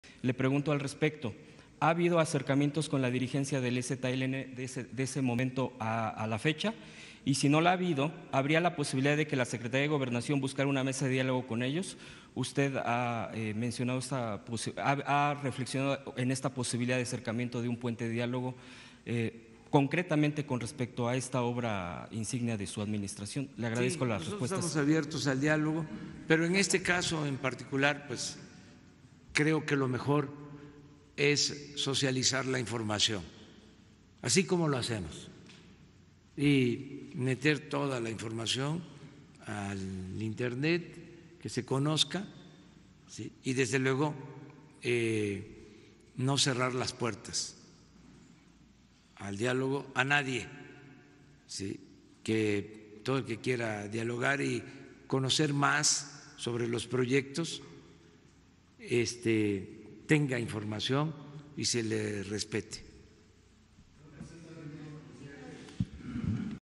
¿Ha habido acercamientos con la dirigencia del EZLN de ese momento a la fecha?
Le pregunto al respecto. ¿Ha habido acercamientos con la dirigencia del EZLN de ese momento a la fecha?

Y si no la ha habido, ¿habría la posibilidad de que la Secretaría de Gobernación buscara una mesa de diálogo con ellos? Usted ha mencionado esta posibilidad, ha reflexionado en esta posibilidad de acercamiento de un puente de diálogo, concretamente con respecto a esta obra insignia de su administración.

Le agradezco las respuestas.

Sí, nosotros estamos abiertos al diálogo, pero en este caso en particular creo que lo mejor es socializar la información, así como le hacemos, y meter toda la información al internet, que se conozca, y desde luego no cerrar las puertas al diálogo, a nadie, que todo el que quiera dialogar y conocer más sobre los proyectos tenga información y se le respete.